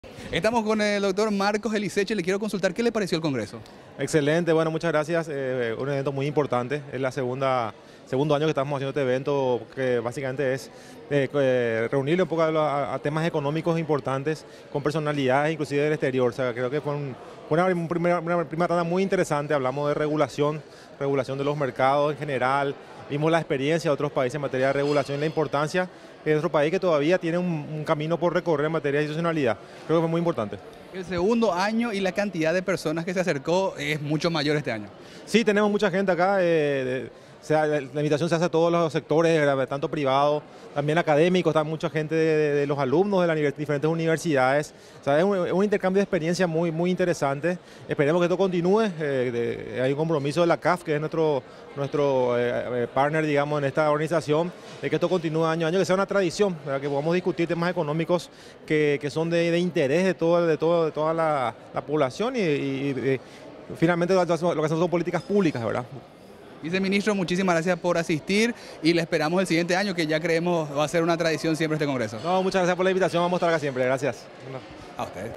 Entrevista Marco Elizeche
Segundo Congreso Paraguayo de Investigadores, Productores y Hacedores de Políticas Económicas